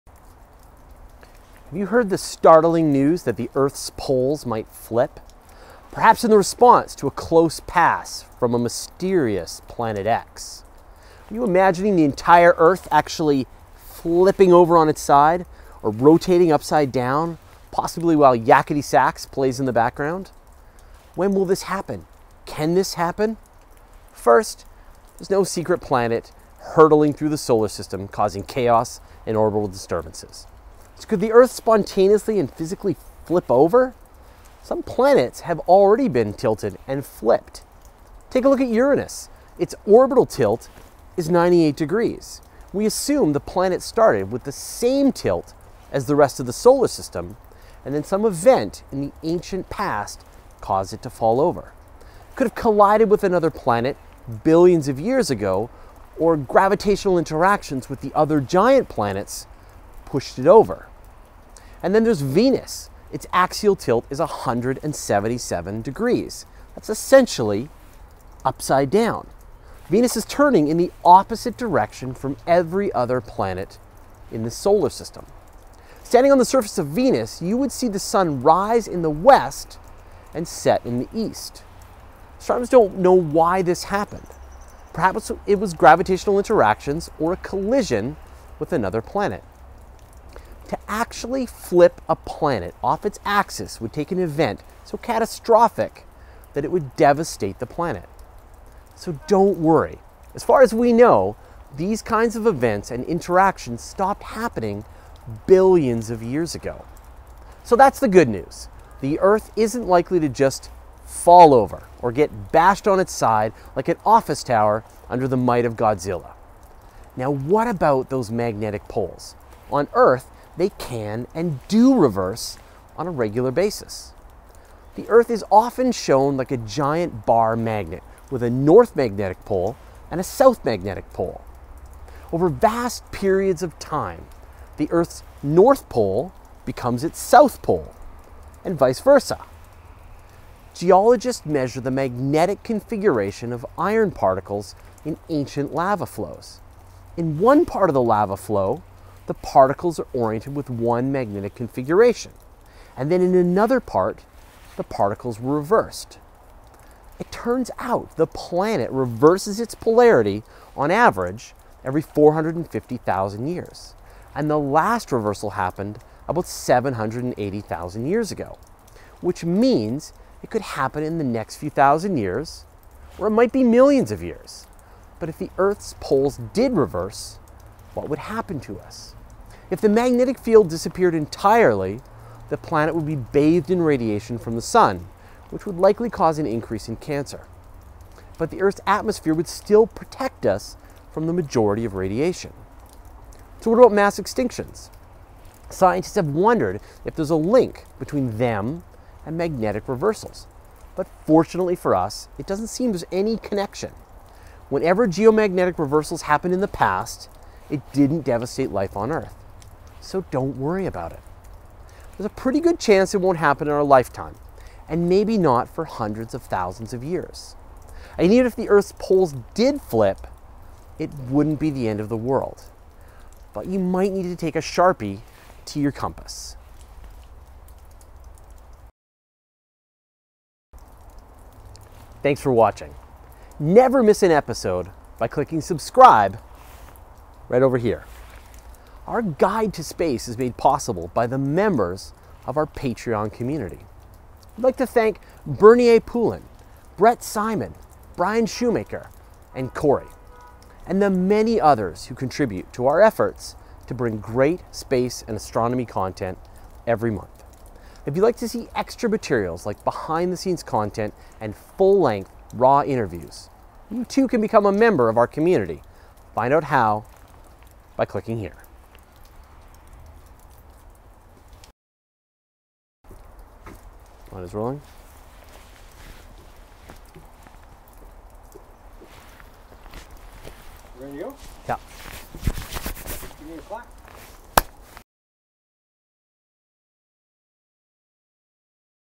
What Happens When The Poles Flip? Understanding the Earth's Magnetic Field Reversal
Have you heard the terrifying news that the Earth's poles are going to flip? What does "flipping" mean? And if the Earth's poles do flip, are we in any danger?